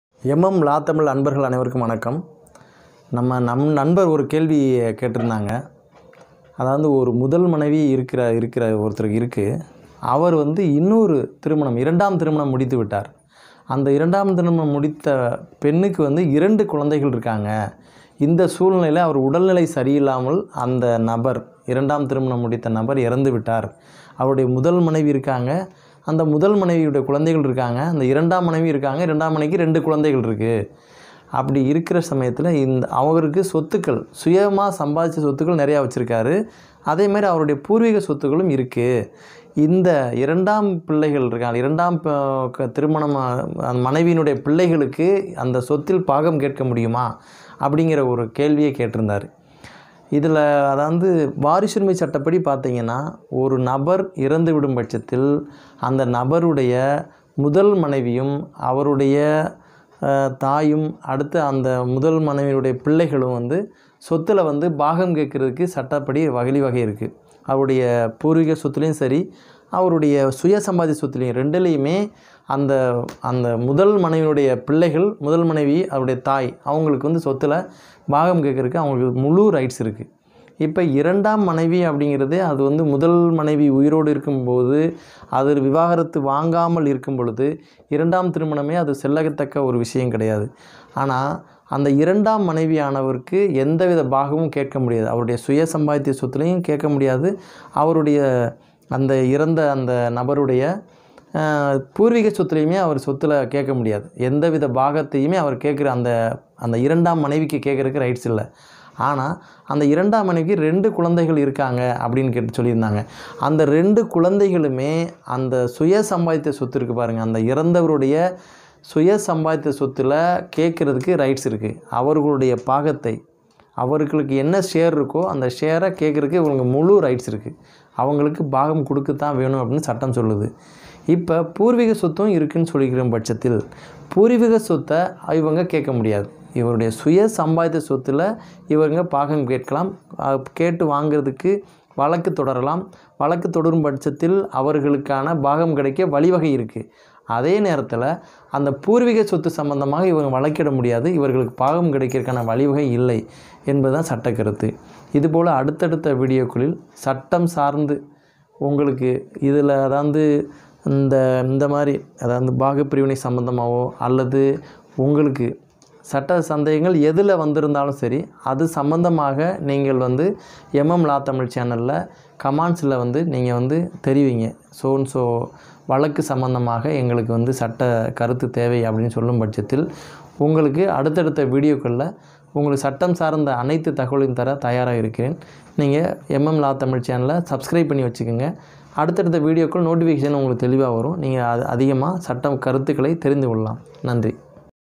இரண்டாம் மனைவிக்கும் அவருக்கு பிறந்த குழந்தைகளுக்கும் சொத்தில் பங்கு உண்டா? சட்டம் அறிவோம்!
2 nd wife and her son can get partition from father property
இரண்டாம் மனைவிக்கும் அவருக்கு பிறந்த குழந்தைகளுக்கும் சொத்தில் பங்கு உண்டா? என்பது பற்றி பார்ப்போம்

சட்டம் சார்ந்த சந்தேகங்களுக்கு விளக்கம் பெற வழக்கறிஞர் மணிகண்டன்